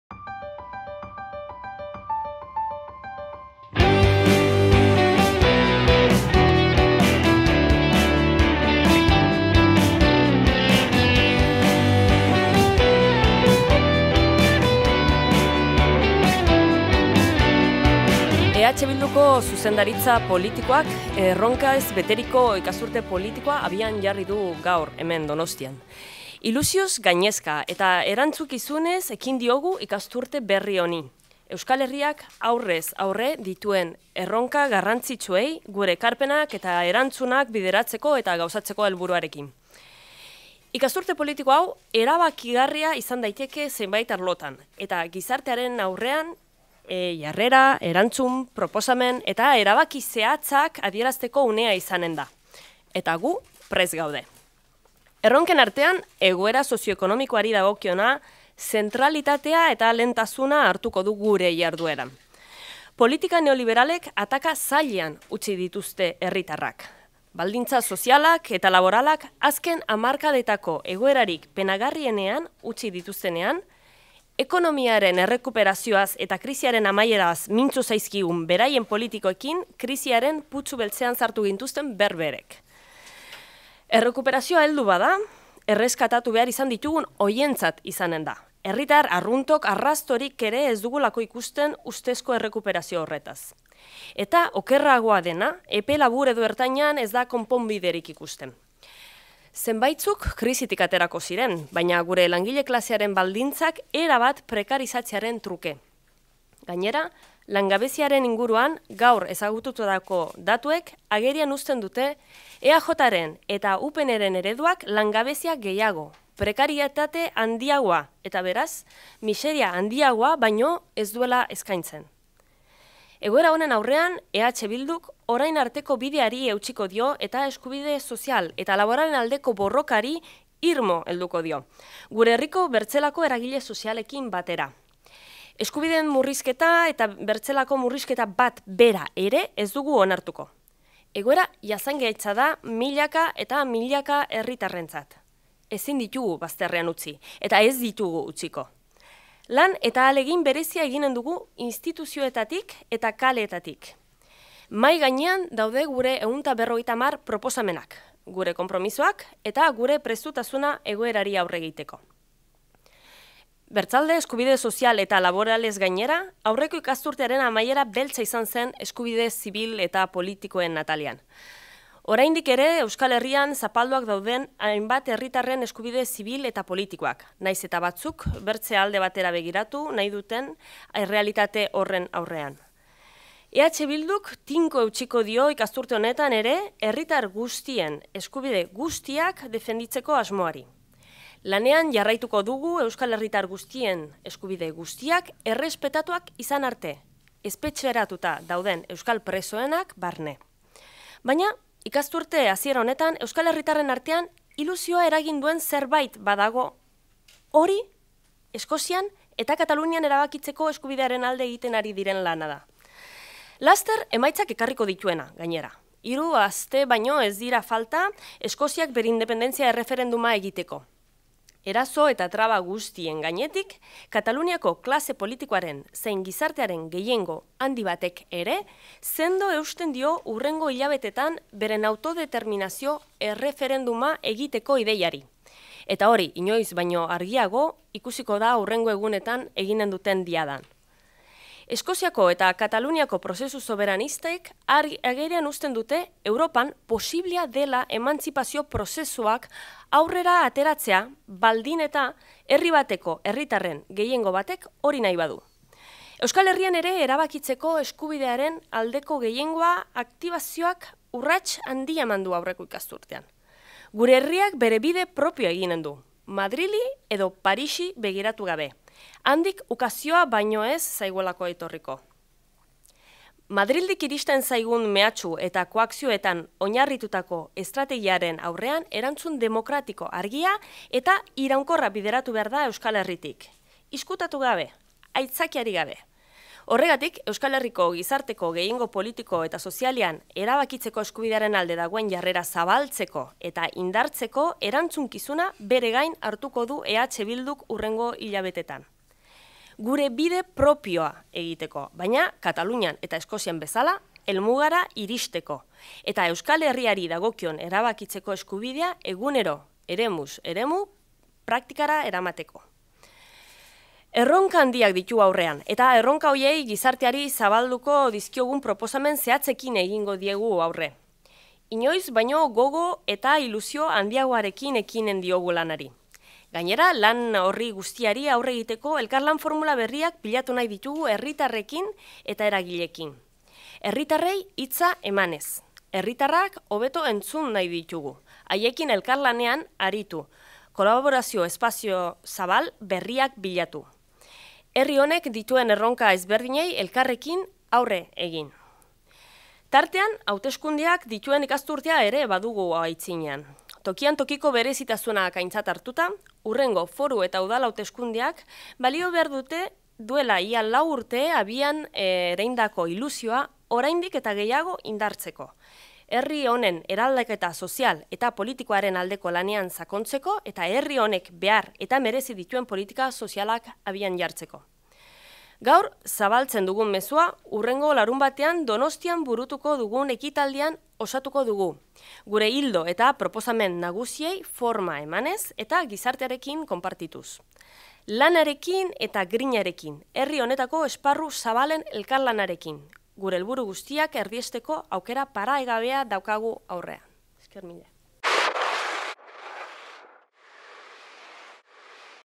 Euskal gizarteak neurriko erantzuna emango dio Espainiako Gobernuaren amaigabeko ukazioari
EH Bilduko zuzendaritza politikoak erronkez beteriko ikasturte politikoa abian jarri du gaur. Ilusioz gainezka eta erantzukizunez ekin diogu ikasturte berri honi, Euskal Herriak aurrez aurre dituen erronka garrantzitzuei gure ekarpenak eta erantzunak bideratzeko eta gauzatzeko helburuarekin.